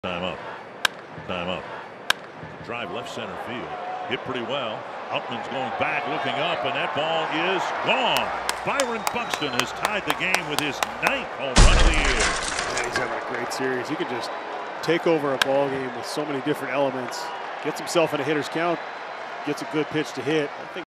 Byron Buxon lifts a solo home run
Byron Buxon lifts a solo home run to left-center field to tie the game at 1 in the 4th inning